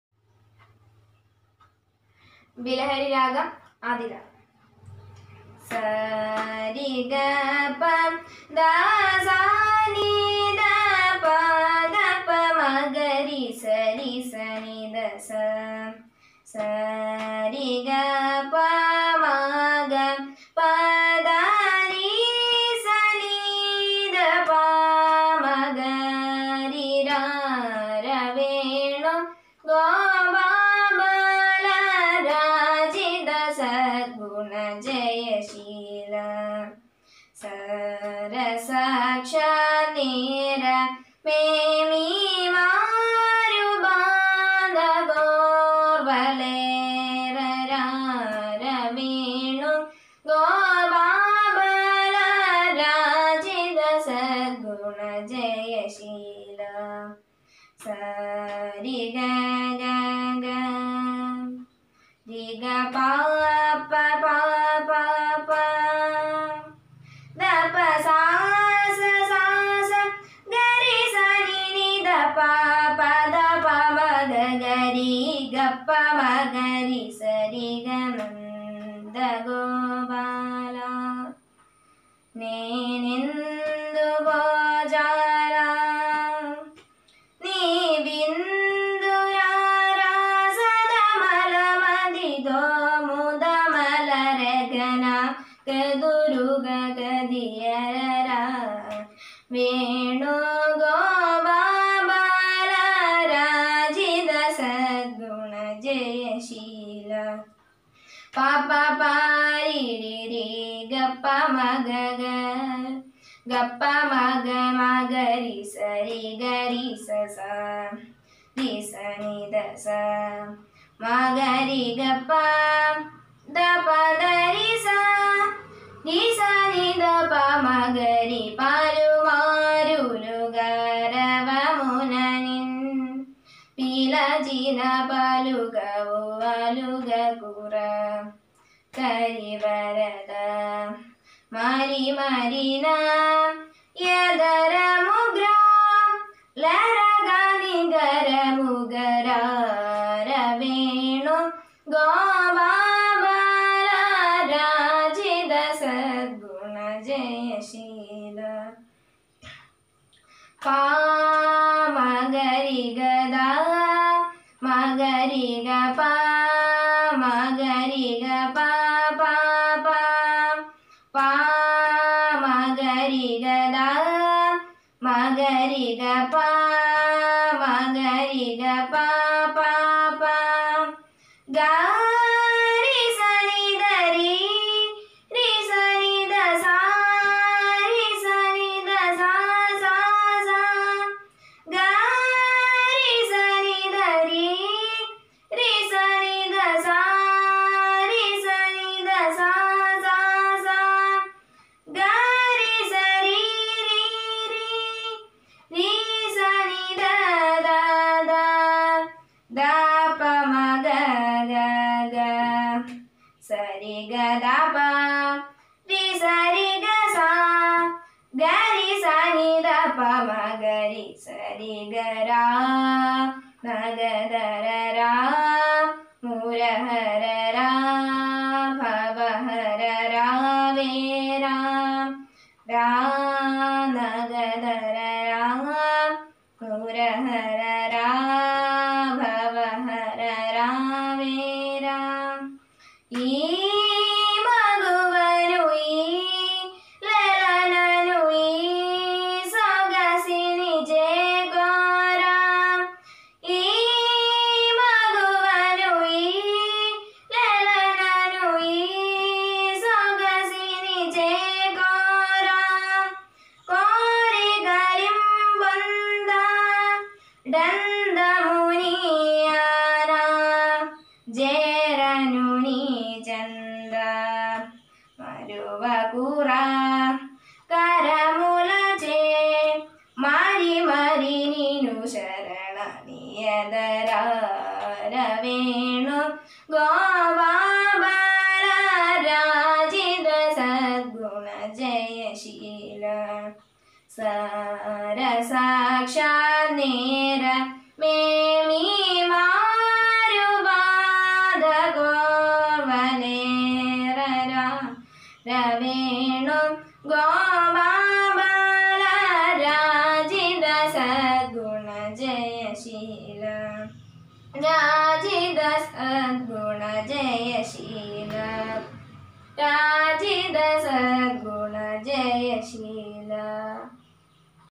നവരാത്രി സംഗീതോത്സവം അഞ്ചാം ദിവസം ആലാപനം കുമാരി അനുപ്രിയ ക്ലാസ്സ്‌ VII.
Vivekananda Public School Kurumulloor